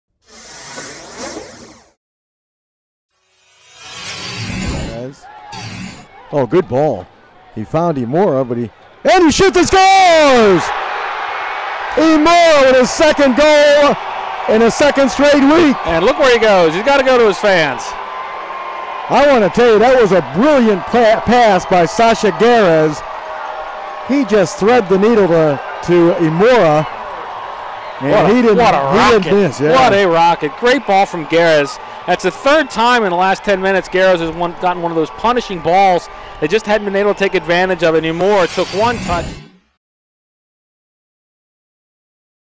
Moment Of Tha Match: Imura Cracks Kickers' Opener
The Richmond Kickers earned a vital three points at home, with Yudai Imura opening the scoring with a terrific finish into the top corner.